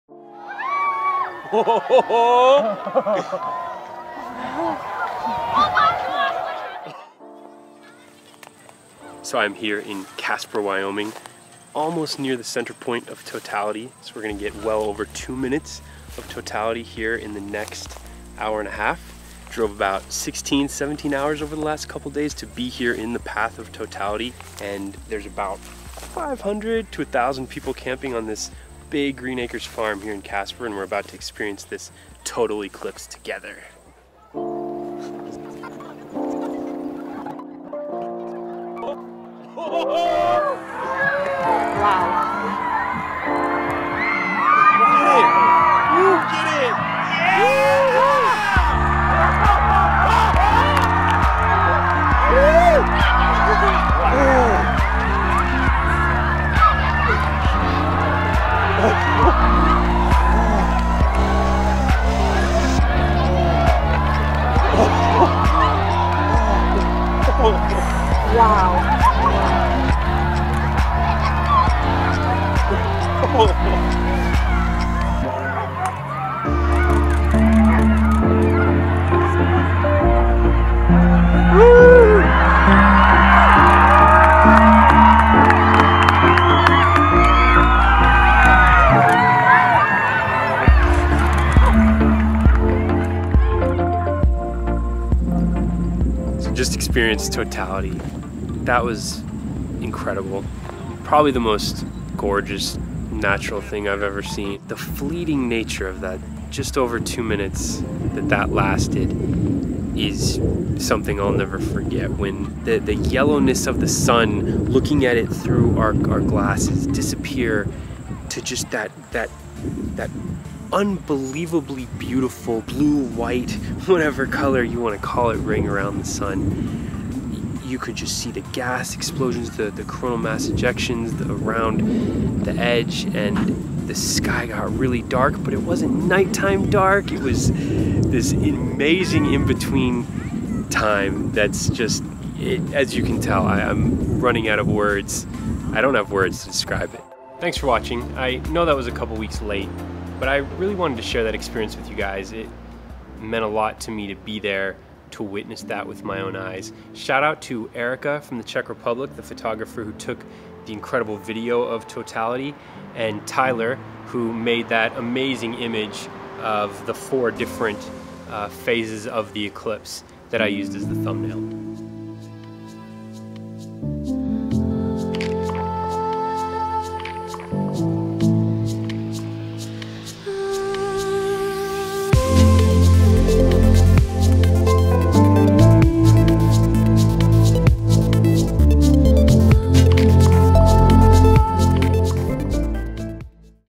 My Trip to a Total Solar Eclipse
I traveled to Casper, Wyoming to experience totality—a complete solar eclipse.

Video by Bryce Plank

How was your 2017 eclipse experience?